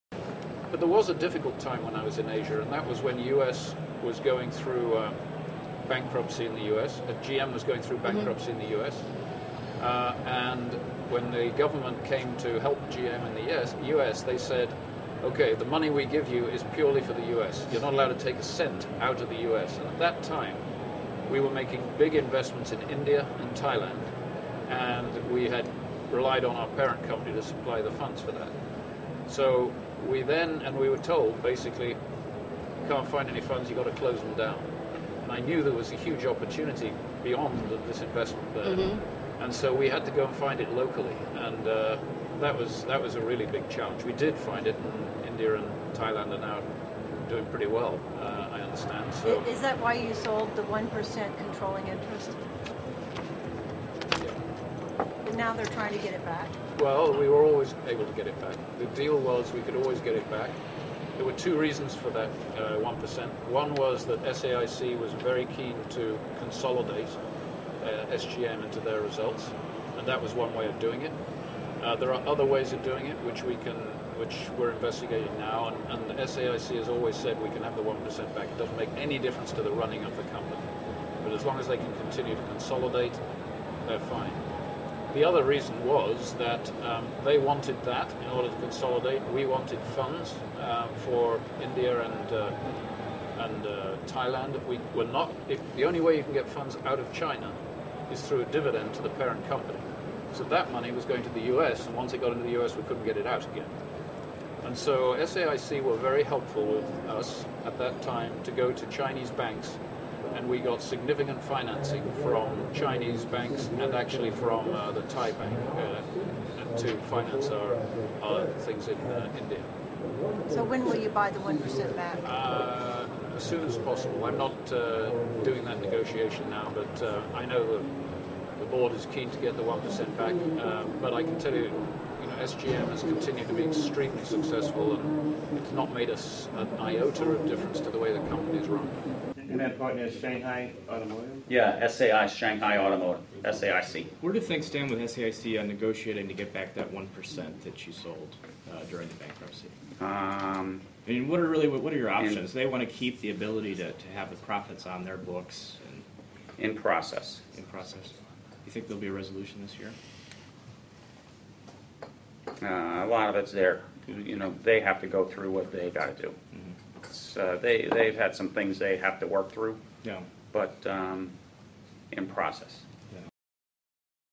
GM-China's one percent controlling interest explained GM-Europe General Motors Frankfurt auto show
GM-China's one percent controlling interest explained on Driving the Nation

Frankfurt, Germany - It's been a concern since it happened. Why did General Motors sell the one percent controlling interest of GM-China to Shanghai Automotive (SAIC)?

Nick Reilly, who heads GM in Europe, used to work in GM-China. Reilly explained why GM-China sold the one percent controlling interest to SAIC. Reilly said that SAIC will allow GM-China to buy the one percent back. It is in the works.

Former securities analyst, Stephen Girsky is the Vice Chairman of General Motors in charge of corporate strategy and business development. Girsky answers the question from another journalist as to when the one percent controlling interest will be re-purchased by General Motors.